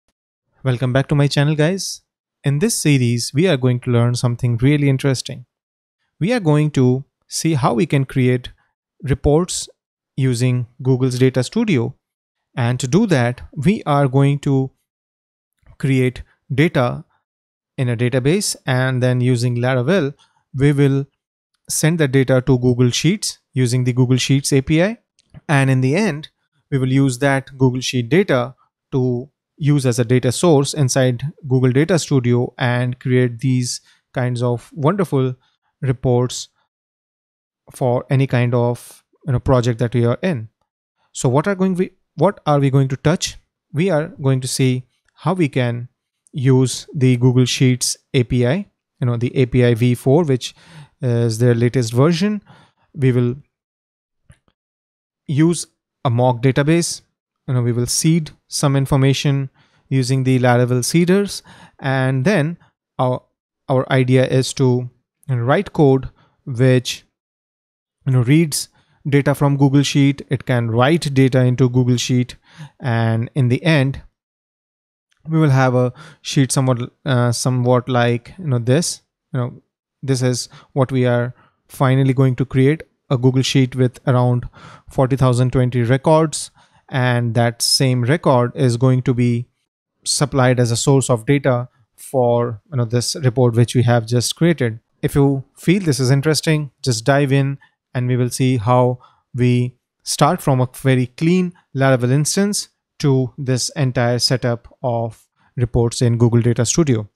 1- Google Data Studio, Sheets API along with Laravel to create reports
In this video, we are going to look at how we can use Google Data Studio to create wonderful reports from any kind of data of our application. We will use Laravel to communicate with Google Sheets using the Google Sheets API v4 to read and write data. And then, we will push data from our database into the Google Sheet and then use that as a data source for our Google Data Studio reports.

This is a very nice way of integrating your data with Google Data Studio. Representing data in form of reports is a very important part of any application. And, if you can play around with the data to form different kinds of reports, then it is really helpful.